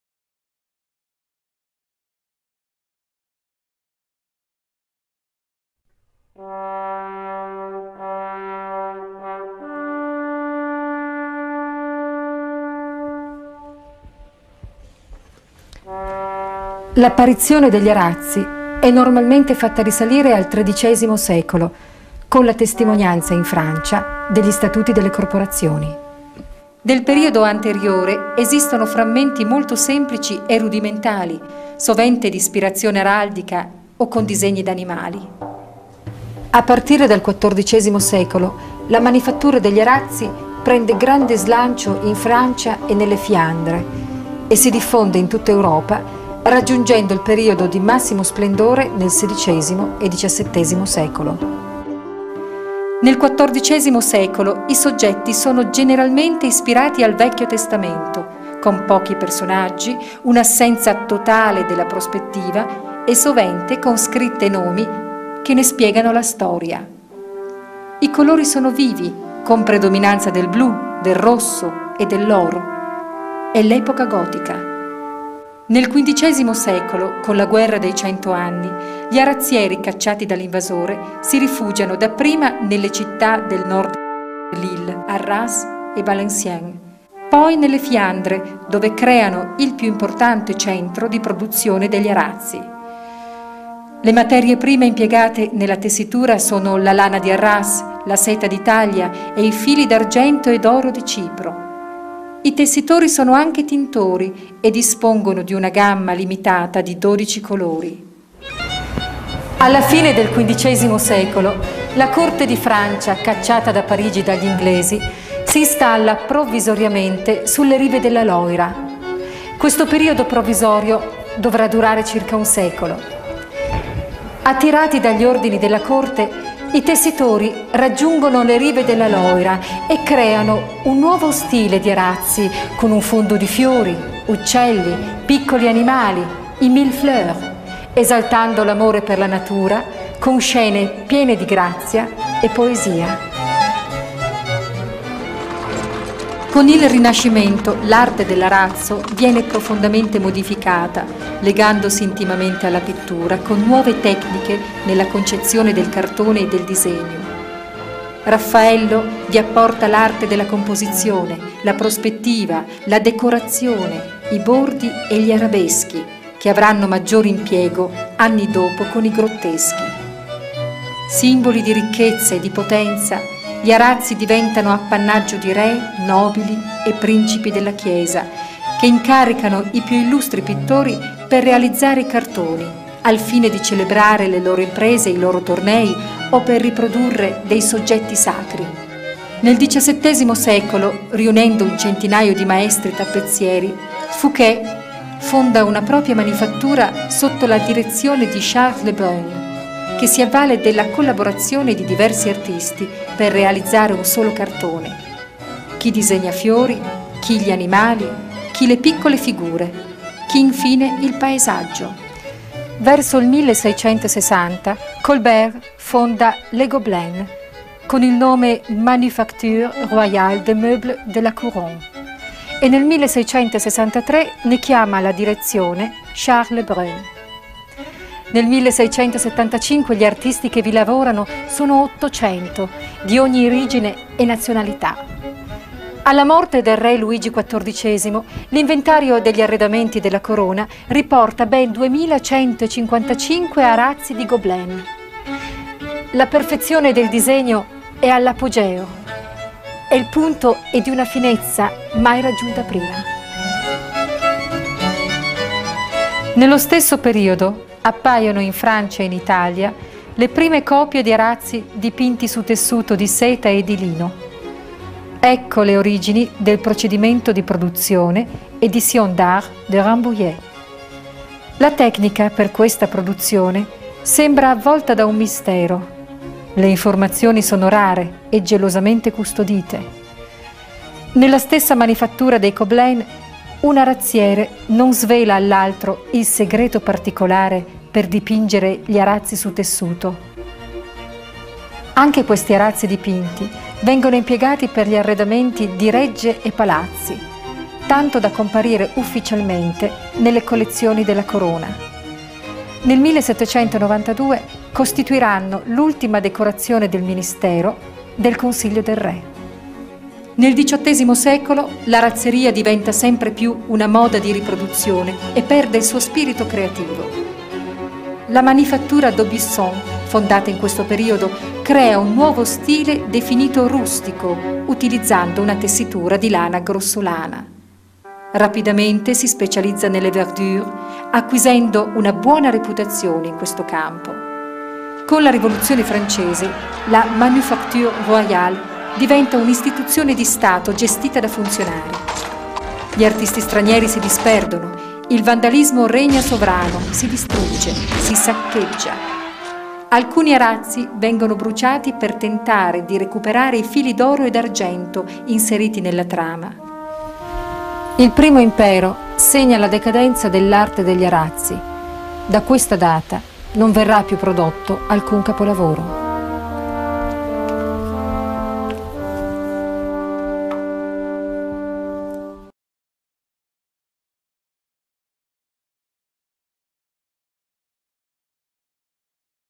STORIA DEGLI ARAZZI by gb rugs
Storia degli Arazzi. Dalle origini al procedimento di produzione degli arazzi Edizion d’art De Rambouillet: Le riproduzioni degli arazzi antichi più famosi del mondo.